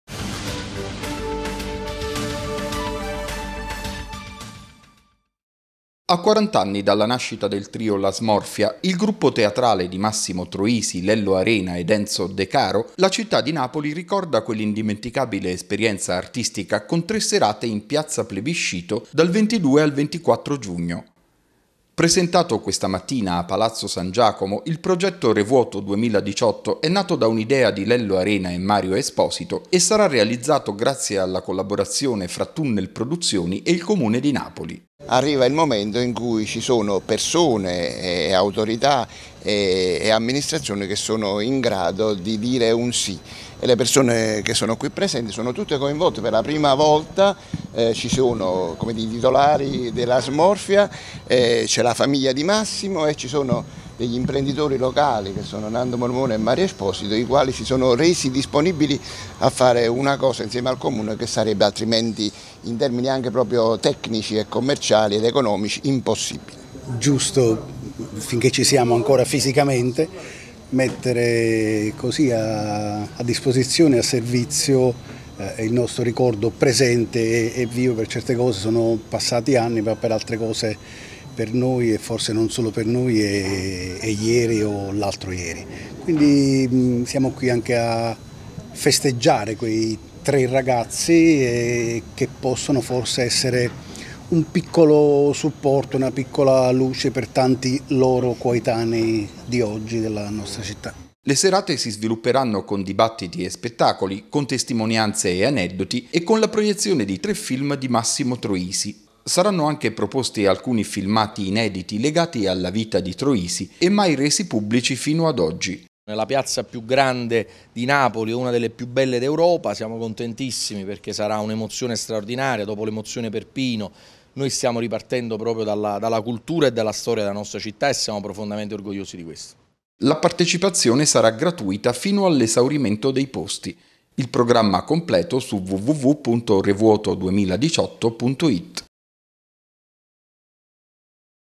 A piazza Plebiscito con Massimo Troisi, «La smorfia» e alcuni rarissimi inediti
La città di Napoli celebra il ricordo del trio «La smorfia», che lanciò sulle scene teatrali - e poi in televisione - Massimo Troisi, Lello Arena ed Enzo Decaro.